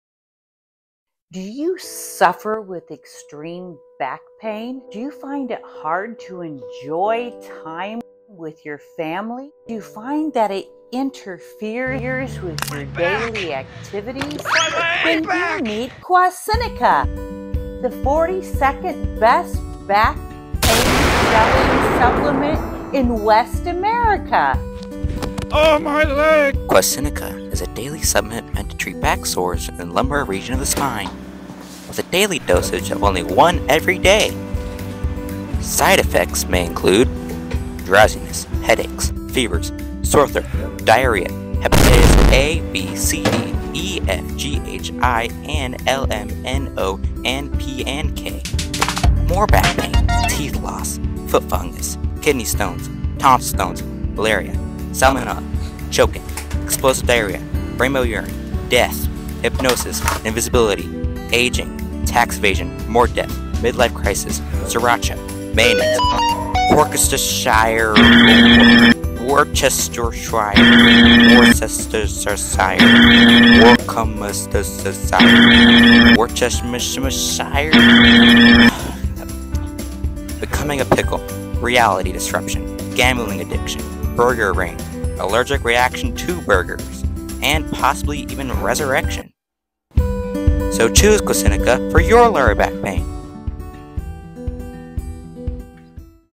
Comedic ads #1 - Quacinica Medical prescription ad
Comedic ads #1

This is a advertisement for quacinica,
Quacinica is not real and this ad is for comedy purposes only

No person was hurt in the making of this all weapons were strictly non functional props

Credits:
Directed and edited by: River R.
Medical professional: Diane
Actress1: Sophia P.
Actor 2: amos P.
Actor 3: River R.
Voice acting by: River R.

Special thanks to Powell pile for acting in our commercial and Diane for being a great actor and for volunteering to act in our commercial.